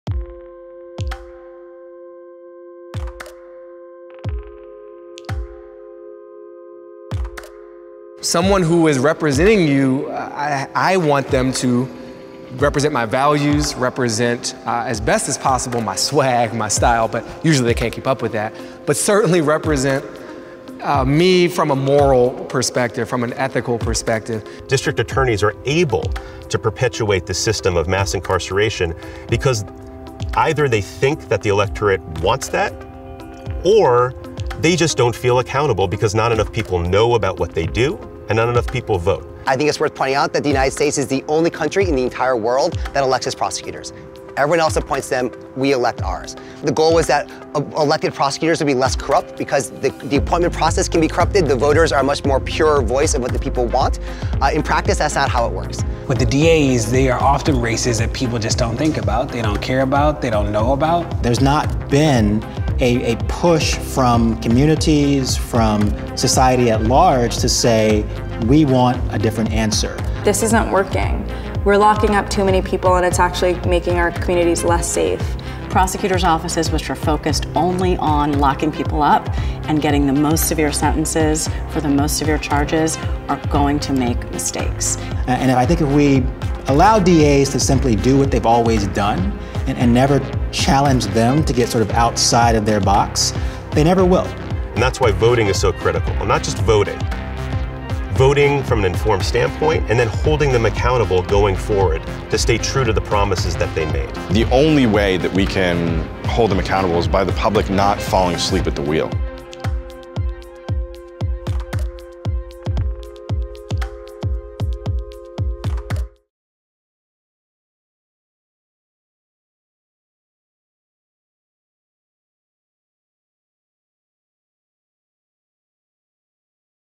The Power of Prosecutors: Accountability
Prosecutors have used their power to pack jails and prisons. And it has taken decades, billions of dollars, and thousands of laws to turn the United States into the largest incarcerator in the world. But did you know that prosecutors also have the power to dismantle this machine — even without changing a single law?

Almost all prosecutors in America are elected officials. And voters across the United States — in red and in blue states alike — strongly prefer elected prosecutors who are committed to reducing incarceration, ending racial disparities, and being fully transparent. This video explains how voters can hold prosecutors accountable because power concedes nothing without a demand.

This video is part of a series, presented by the ACLU Campaign for Smart Justice and Brooklyn Defender Services, shows how prosecutors can single-handedly transform the broken American criminal justice system.

The videos feature Deray McKesson (activist and Black Lives Matter founder), Nina Morrison (The Innocence Project), Baratunde Thurston (Author and Comedian), Adam Foss (a former prosecutor), Scott Hechinger (Brooklyn Defender Services), John Pfaff (professor and author of Locked In), Josie Duffy-Rice (Fair Punishment Project), and Brandon Buskey (ACLU).

To sign up for ACLU action alerts on ending mass incarceration, text SMARTJUSTICE to 82623.